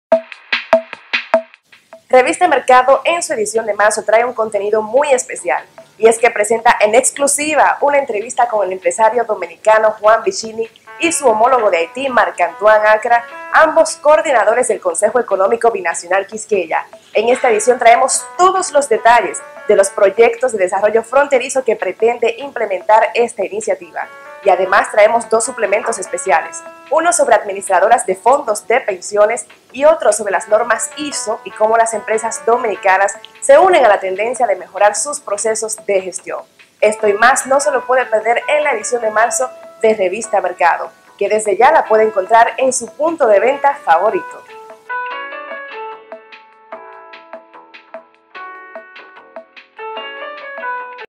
No te pierdas la edición marzo 2015 de Revista Mercado
Mercado Media Network presentó en su edición de marzo de 2015 de Revista Mercado, y de forma exclusiva, el proyecto de desarrollo fronterizo entre República Dominicana y Haití, denominado Consejo Económico Binacional Quisqueya, en el cual sus promotores, el empresario dominicano Juan Vicini y su homólogo haitiano, Marc Antoine Acra, detallan en una entrevista el ambicioso plan.